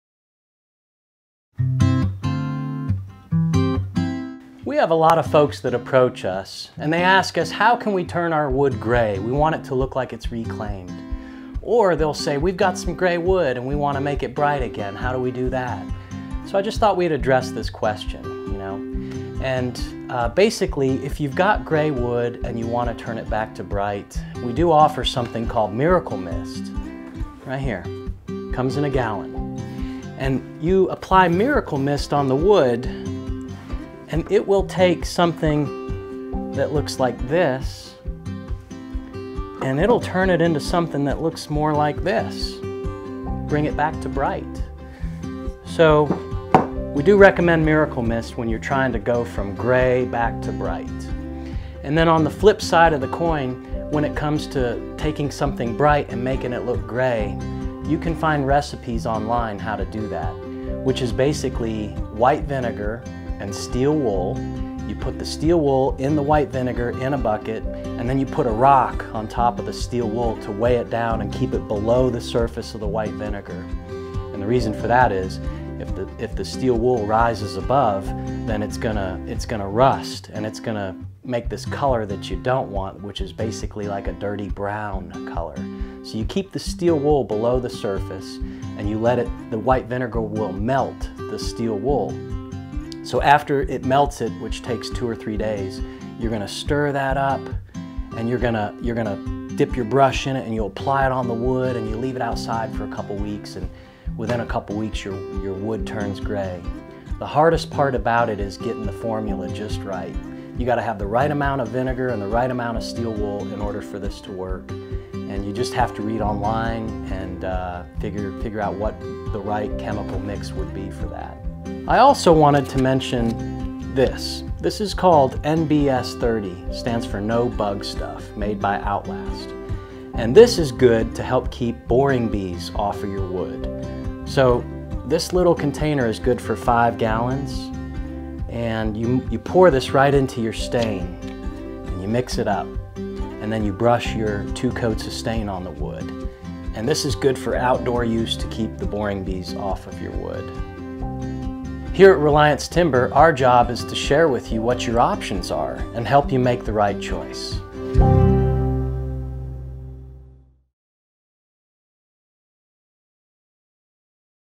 Reliance Timber: How to Brighten and Distress Your Wood
Gary Campbell chats about how to turn your wood gray and go for the reclaimed effect and how to do the opposite and make it bright again.